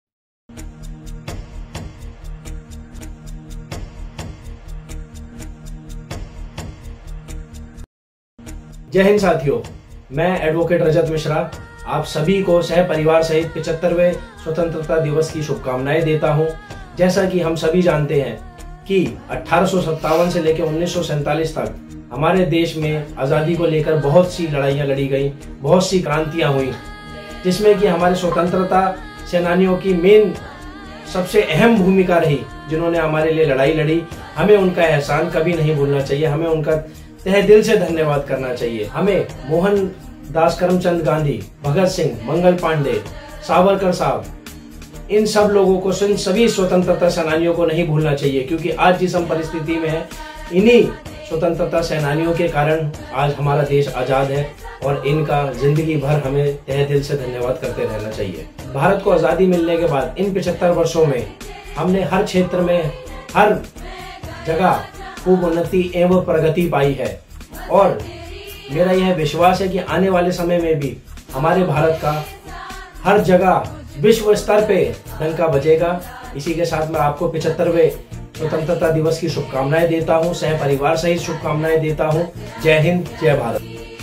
Rajat Mishra | Har Ghar Tiranga | Happy 75th Independence Day | Newslive18 | Dilshad Garden India
Rajat Mishra || Wishing || Happy 75th Independence Day || Newslive18 || Dilshad Garden || India
Wishing you all a very Happy 75th Independence Day Rajat Mishra